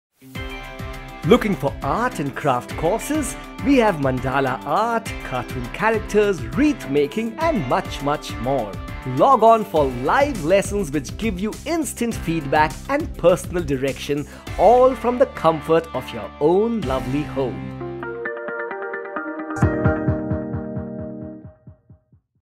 Enhance Art & Craft Skills Online With FSM Buddy
Looking for art & craft courses?
With FSM Buddy’s hearty variety of art classes you can learn a vast range of creative skills!
Learn Mandala Art, cartoon characters, wreath making and so much more. All of our online classes are live, taught by expert teachers and trained professionals in the field. With courses for Mandala Art, Marble Painting, Mosaic Painting, Doodle and Cartoon Creation, Sketching, etc., your artistic dream-come-true moment is only a click away at FSMBuddy.